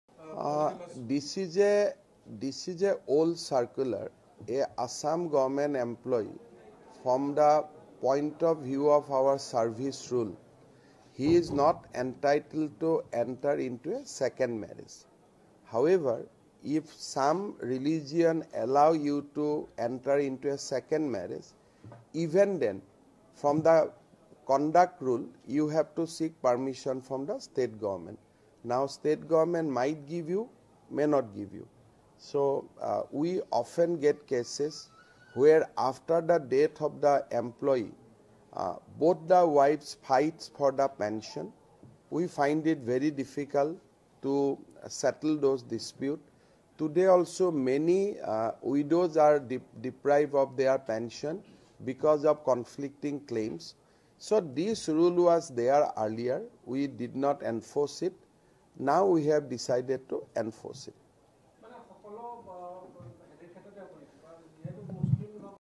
No 2nd Marriage For Assam government employees Without Permission
Even if a religion allows you to have a second marriage, then also you have to seek state government's permission. Assam government employees cannot marry for the second time, if their spouse is alive, without the government's permission, even if they are allowed to by their personal law.